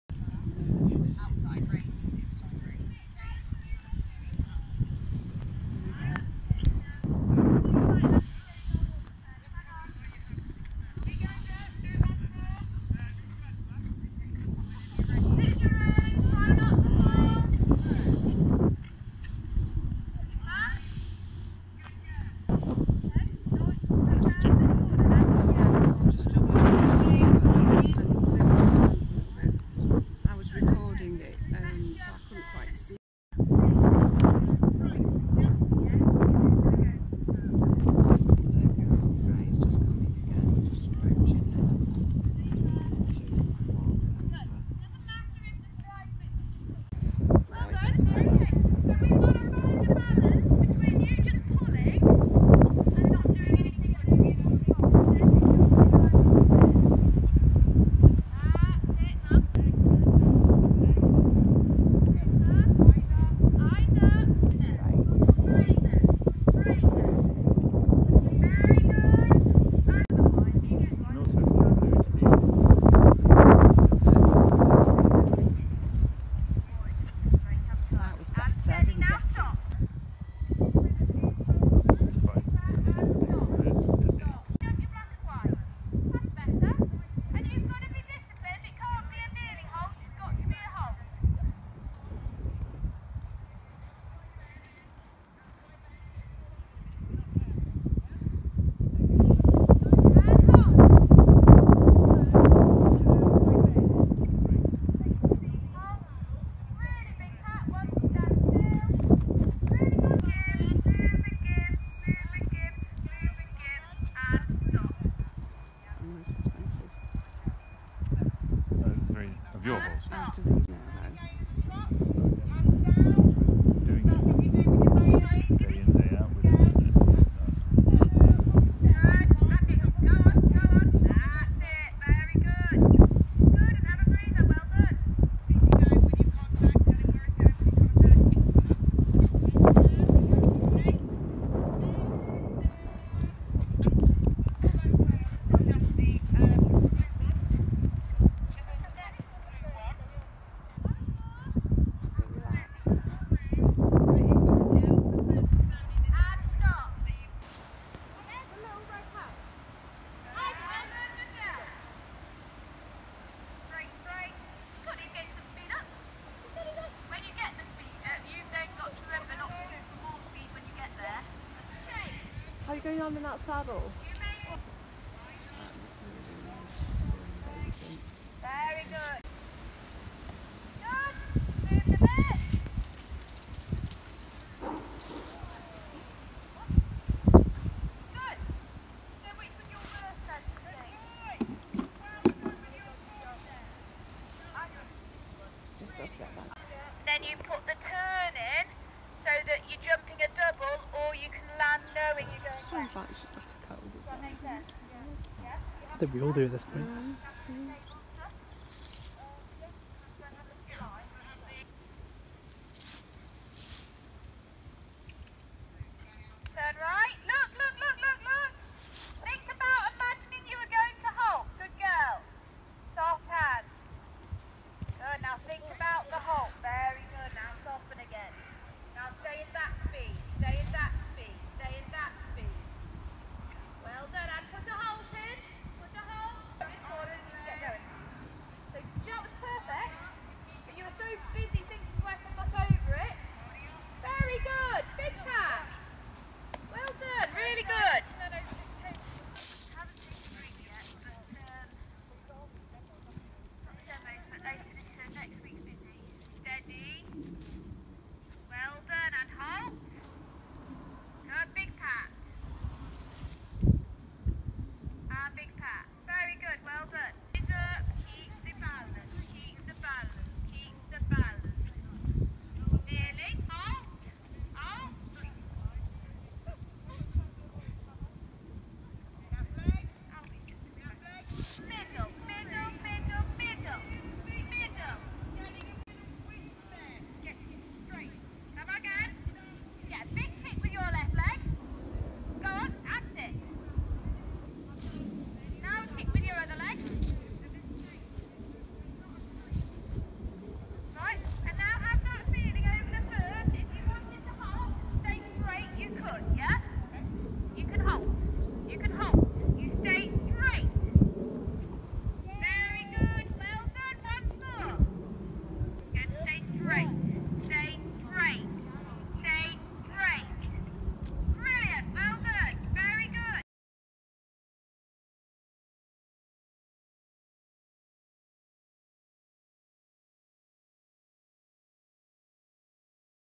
Tranquil Soldier XC Clinic Ros Canter, 11th November 2018, Somerford Park
Our XC clinic with Ros Canter! What a lovely lady. Including some mistakes in here. Sorry about the wind noise. Thanks to James for recording!